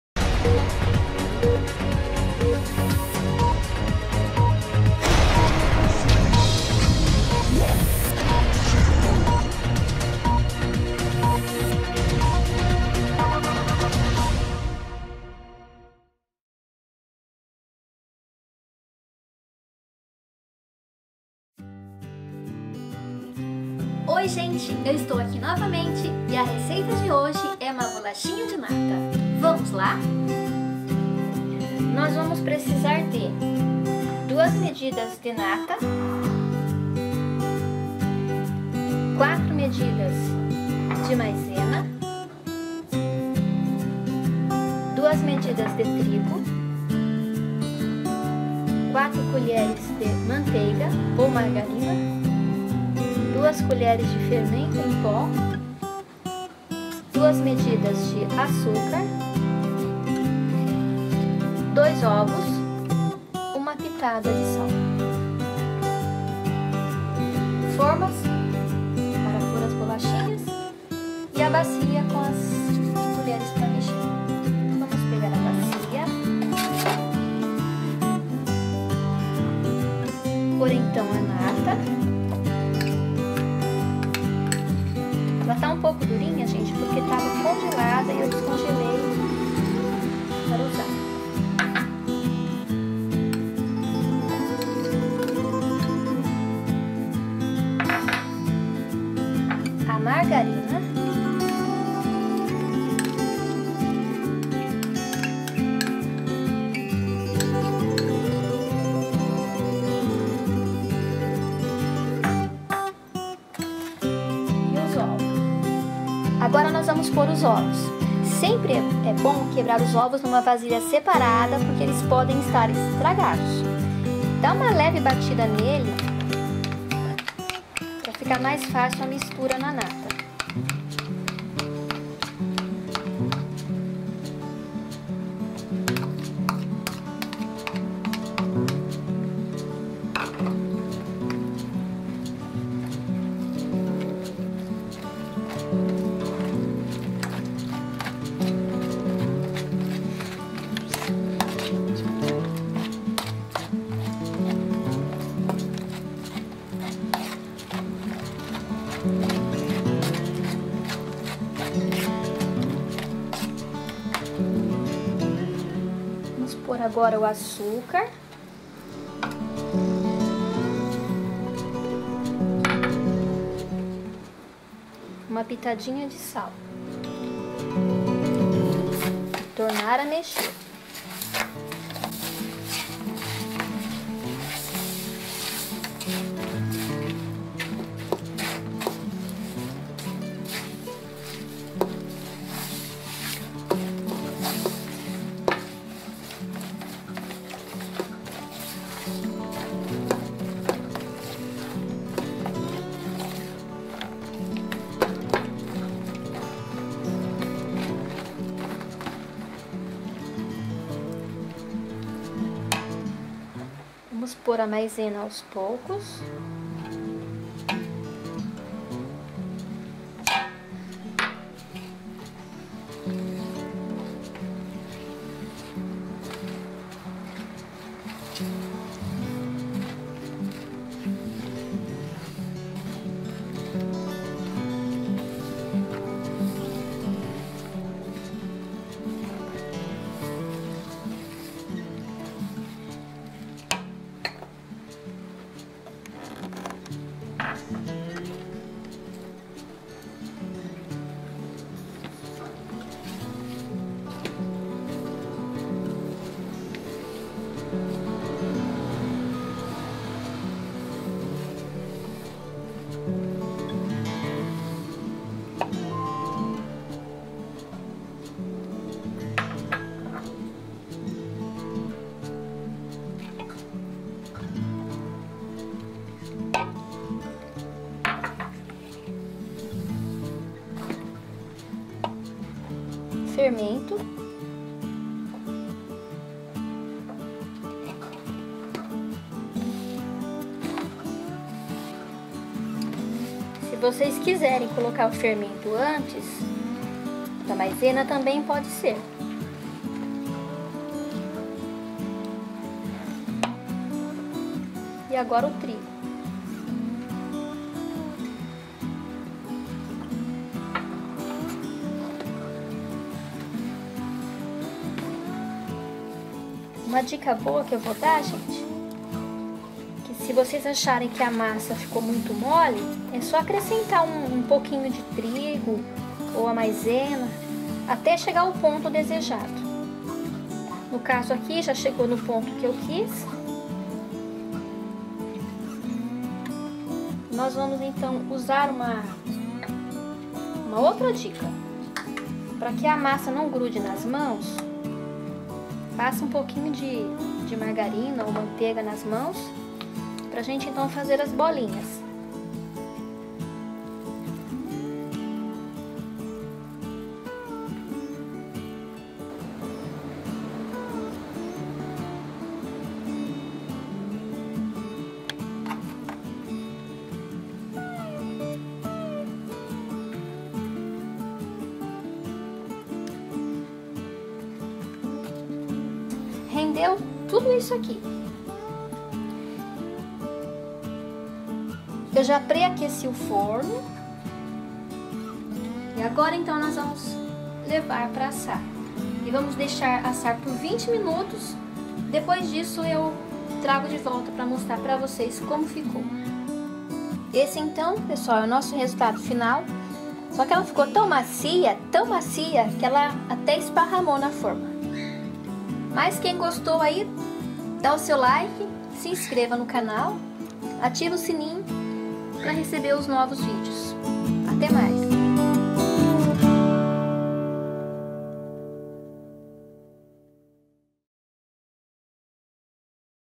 Vida na roça - Bolachinhas de nata.
Bolachinhas de nata com café, não é uma tentação? Aprenda nesse vídeo como fazê-las. Com certeza vocês vão gostar. Aviso aos diabéticos: Se quiser menos doce, logicamente diminua o açúcar.

Ingredientes:
02 medidas de Nata
04 medidas de maizena
02 medidas de trigo
04 colheres de manteiga
02 colheres (sopa) de fermento em pó
02 medidas de açúcar.
02 ovos
01 pitada de sal
OBS.: Uma medida equivale a uma xícara de chá.